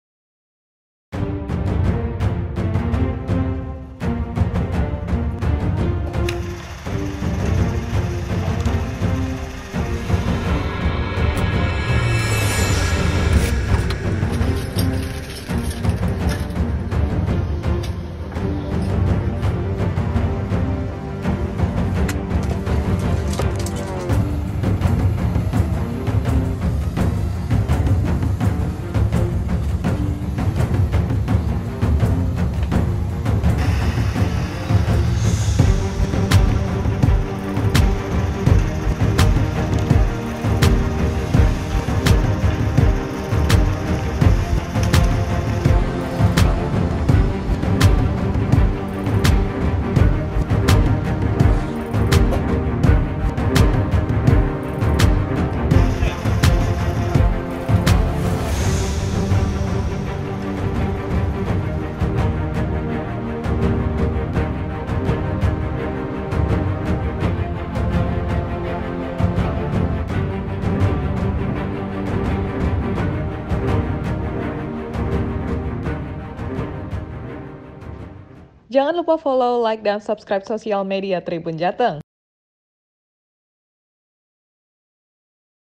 Detik-detik Tentara Rusia Pasang Bendera Kemenangan Setelah Kalahkan Ukraina
Kementerian Pertahanan Rusia menunjukkan video rekaman
Militer Rusia merayakan kemenangan.

Mereka merayakan kemenangandi Kota Belegrovka.